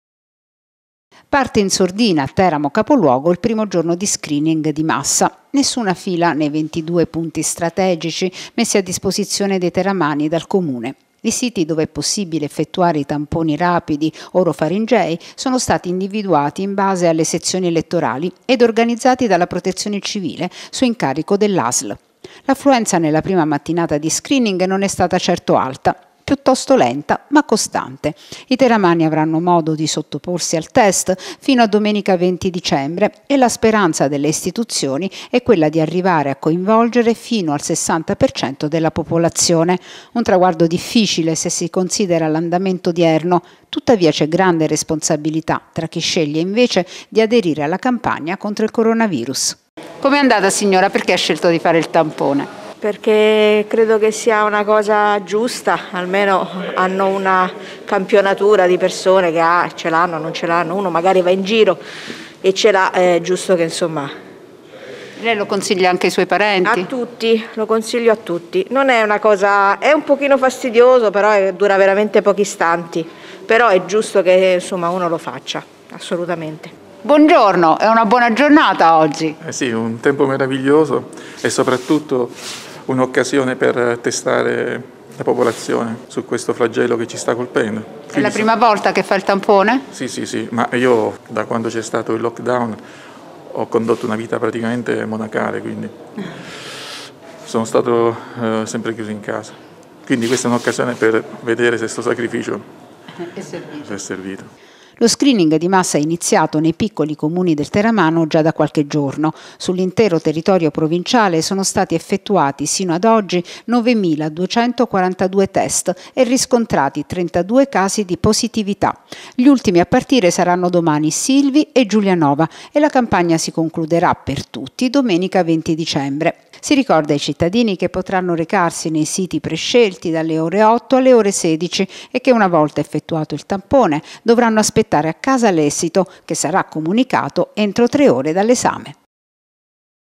Teramo. Scarsa l'affluenza nel primo giorno di screening di massa
E' bassa l'affluenza dei teramani nel primo giorno di screening di massa a Teramo capoluogo. Nessuna fila ai 22 siti predisposti dal Comune e sarà difficile raggiungere il traguardo ambizioso del 60% della popolazione testata. Fino ad oggi in tutta la provincia teramana sono stati effettuati 9.300 test e riscontrati 32 casi di positività.